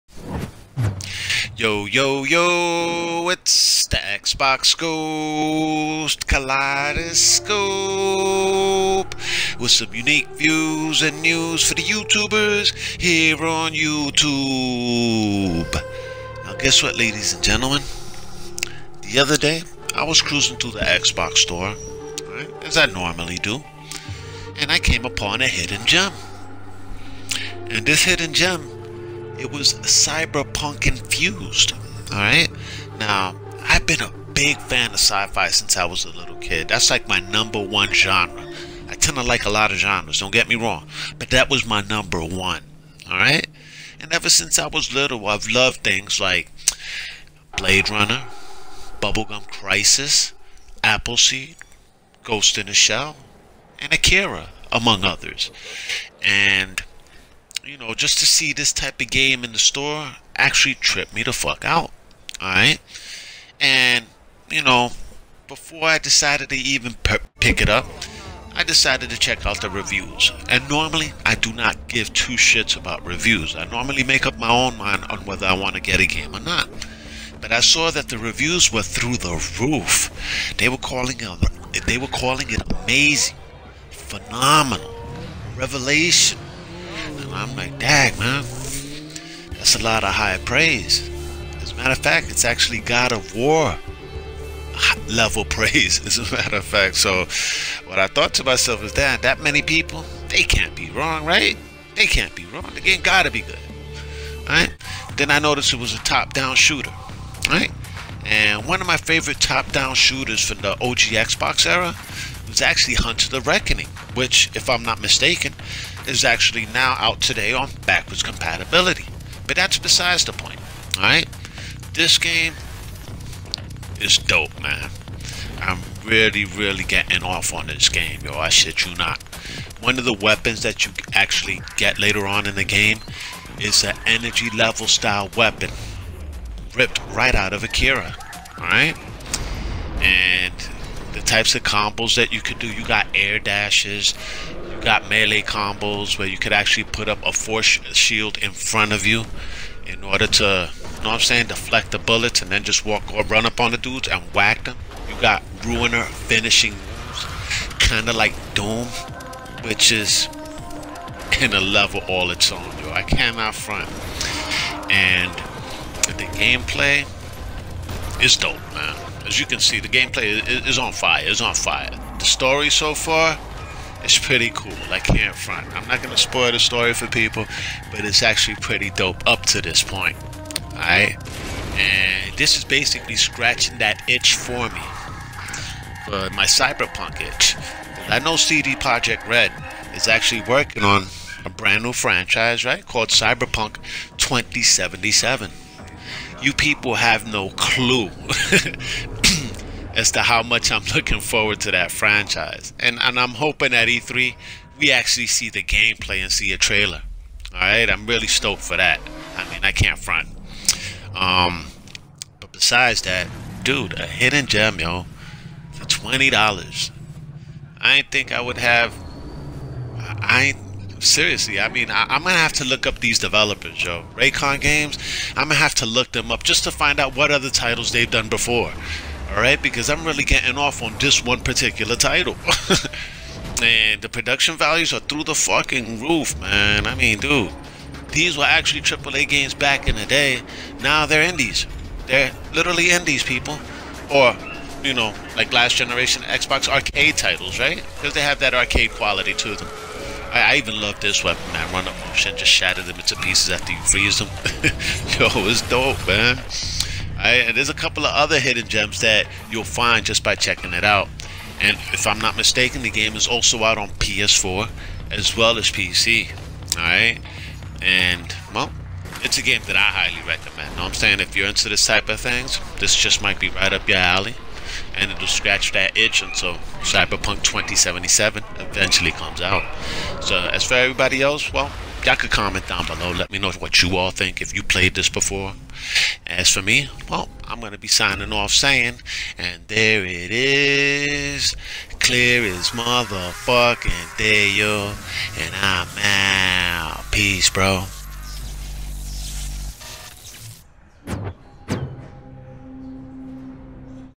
A CYBERPUNK INFUSED HIDDEN GEM
If you are into Cyberpunk or isometric shooters, then there is a good chance this may be right up your alley.